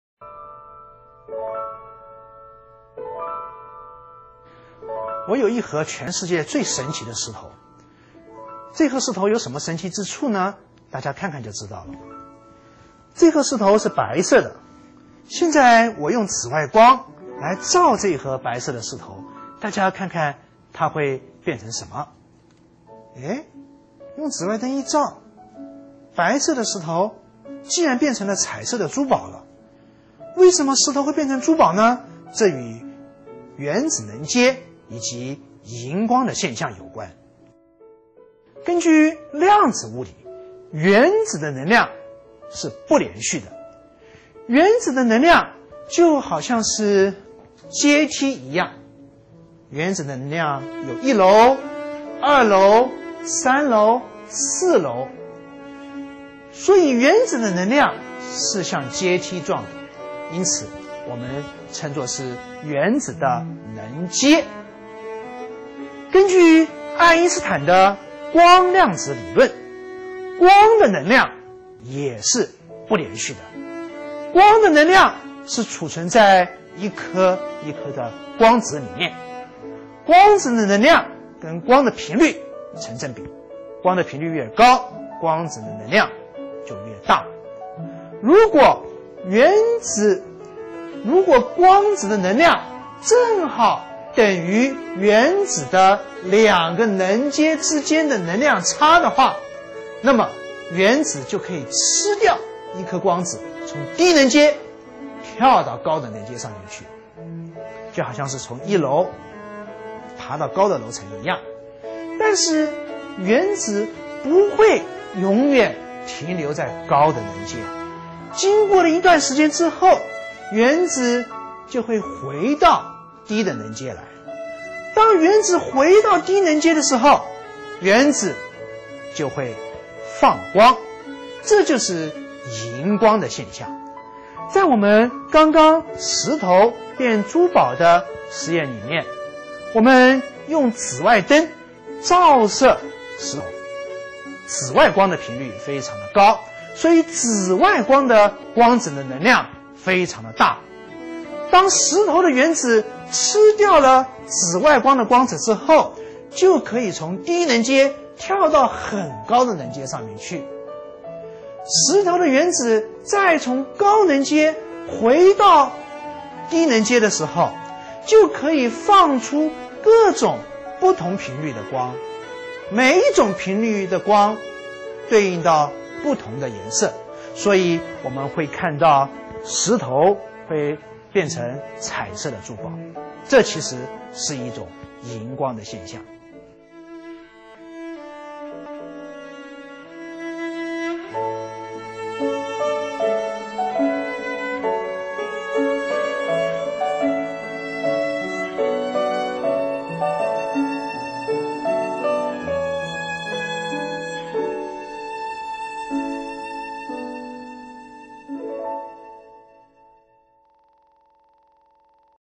石頭變珠寶
老師拿出一盒白色的珠子，經紫外燈照射後，白色的珠子居然變成了彩色的珠子。這是一種螢光現象。要解釋螢光現象，我們必須從原子能階談起。原子吸收光子提昇到較高的能階（激發態），但原子在高能階無法久留，過了一段時間後就會回到低能階。原子從高能階回到低能階會放光，這就是螢光。

紫外光的頻率非常高，因此紫外光的光子具有很高的能量。原子吸收紫外光的光子可提昇到很高的激發態。原子從高激發態回到低能階時會放出各種不同頻率的光。其中有一部份光是可見光，因此白色的珠子就變成了彩色的珠子。